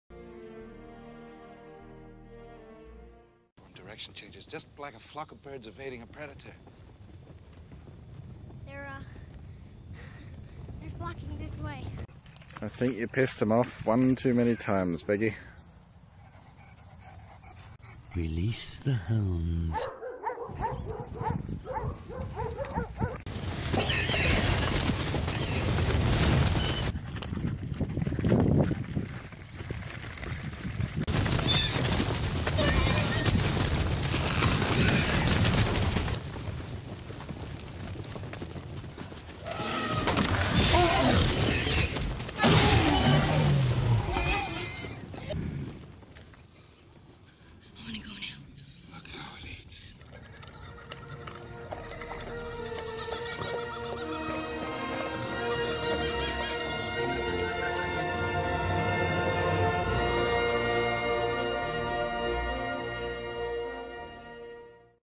Walking a Saint Bernard
A little insight into the daily joys and adventure of walking a dog that weighs as much, if not more than yourself - in this case, my Saint Bernard named Vegas